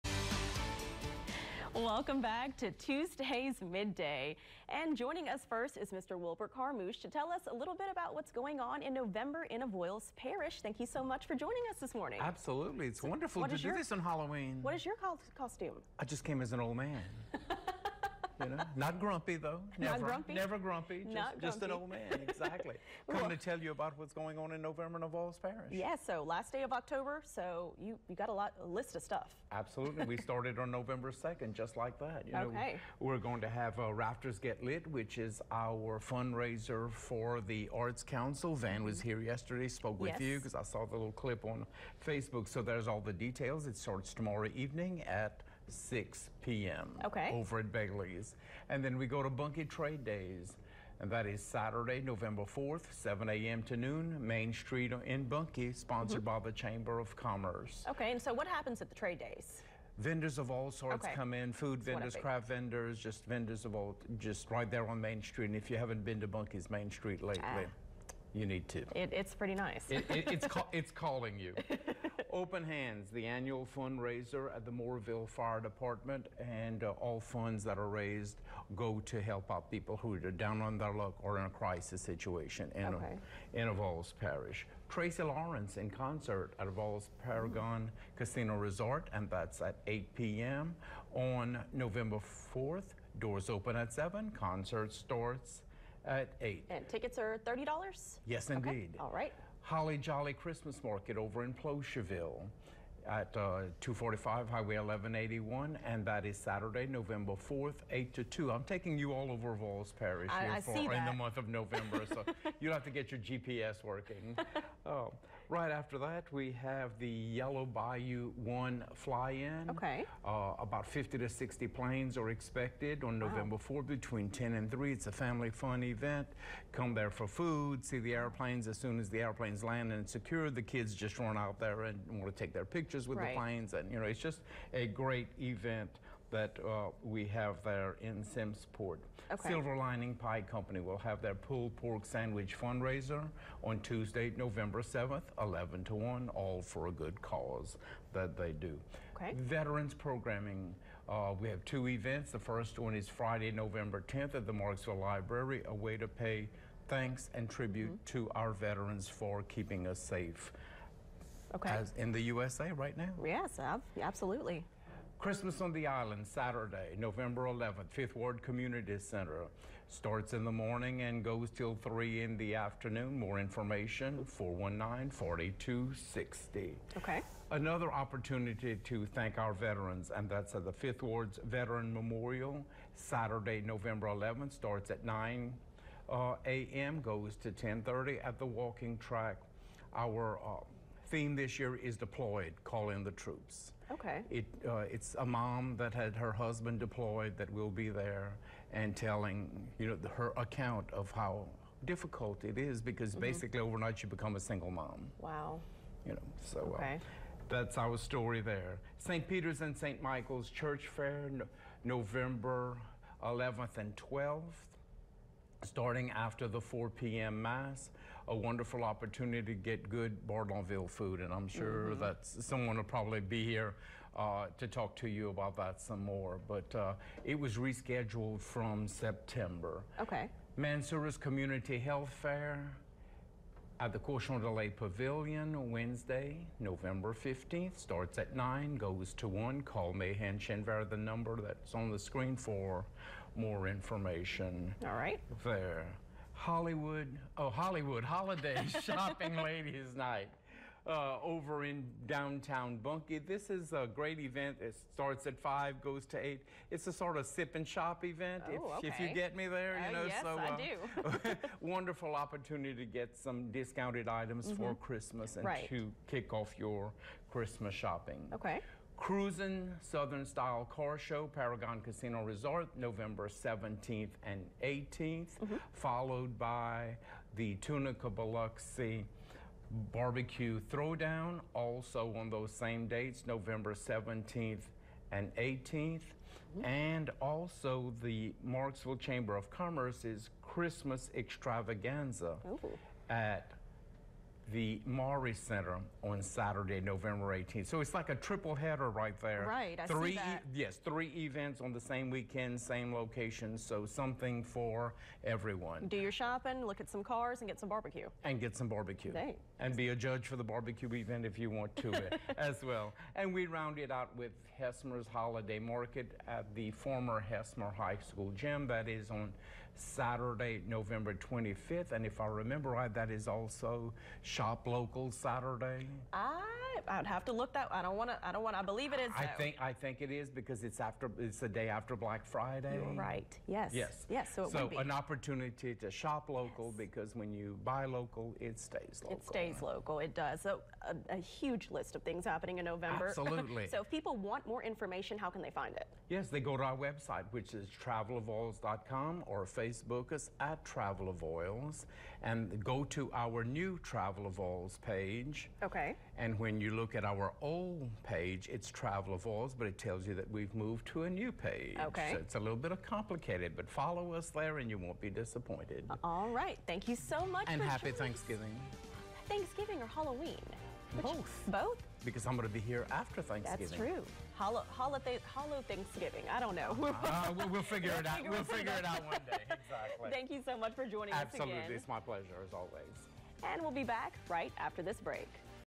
Avoyelles Tourism
Wilbert Carmouche returns to tell us about what's happening in Avoyelles Parish!